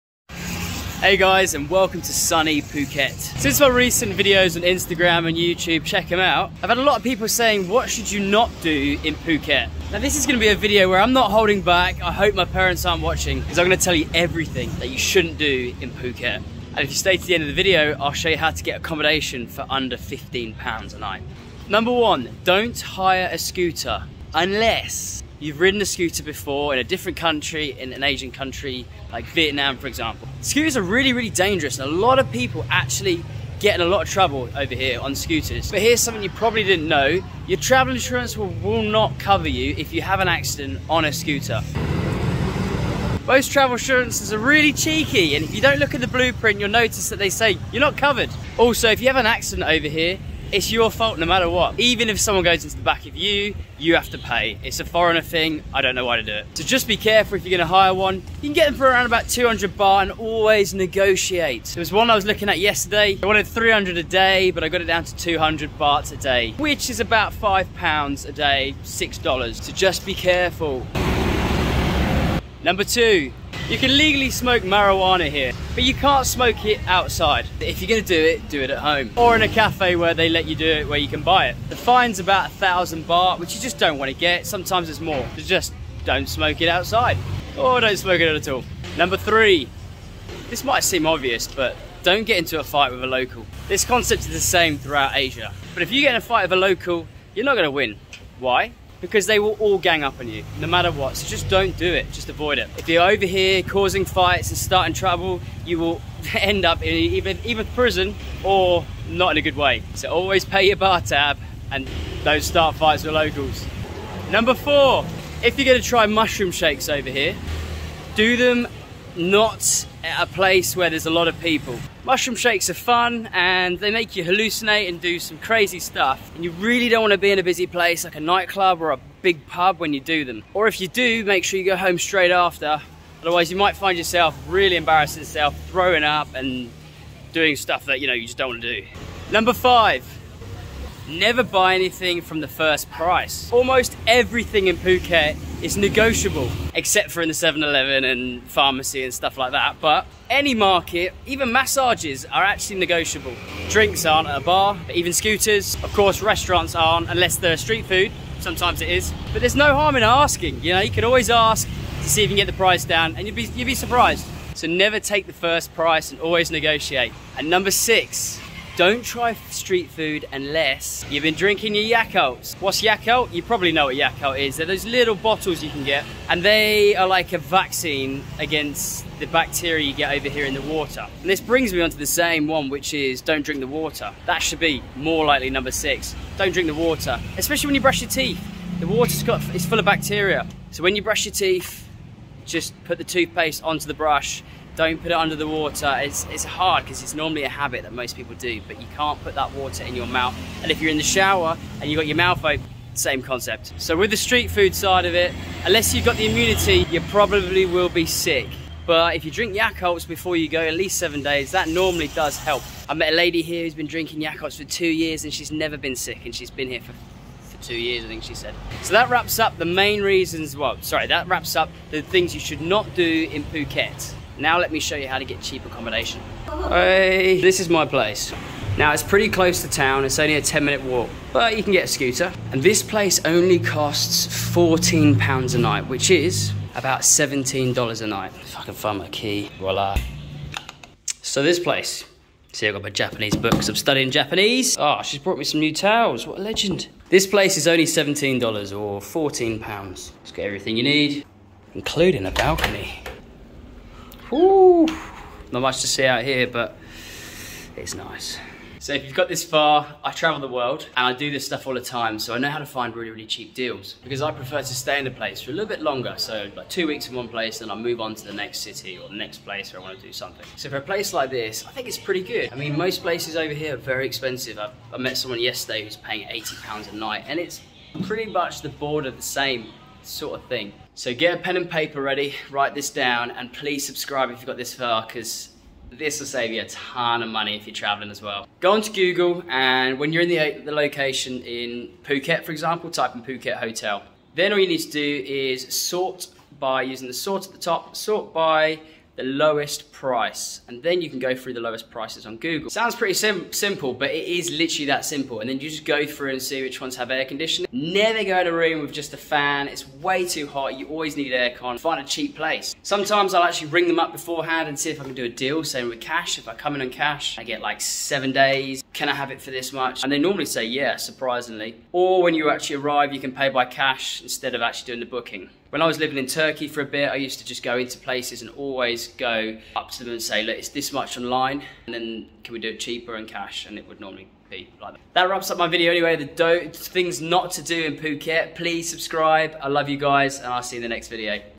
What you Should NOT DO in Thailand, I F*****D Up in Phuket!? 🇹🇭
What you should not do in Phuket Thailand. Welcome to sunny Phuket, today we are going through  what not to do in Phuket or better yet, things not to do in Phuket Thailand. I'll be giving you 6 things you should not do and how to get cheap hotels in Phuket Thailand. I'm not holding back here, telling you from my experiences of being in Phuket more than 3 times, each time spending atleast 3 weeks.

1. Don't ride motorbikes /scooters unless you have a valid license and have checked your travel insurance. Most travel insurance will not cover you iding a scooter in Thailand.

2. Always negotiate the price, never go with the first price when negotiating on almost everything. You can save so much money in Phuket by negotiating.

3. Do not get into street fights with locals, you will not will in Phucket. As with all Asian countries, if you fight a local, you will fight everyone because they will always help one another. Don't be that guy to start fights anyway.

4. Don't hire a jetski without taking a video first of the vehicle, there is a popular scam where they say you have scratched the bottom of the Jetski and will try charge you. So ensure you take a video before hiring a Jetski I'm Phuket, Thailand.

5. Don't drink the tap water, always drink bottled water as this will make you sick. It's a good idea to drink actimel before visiting to prevent you from getting sick from the water and potentially ruining you holiday to Phuket.

Hope this helps anyone on vacation to Phuket in this Phuket Vlog.

Also talk about how to get cheap accommodation in Phuket.

Ho#phuket pe you enjoyed the video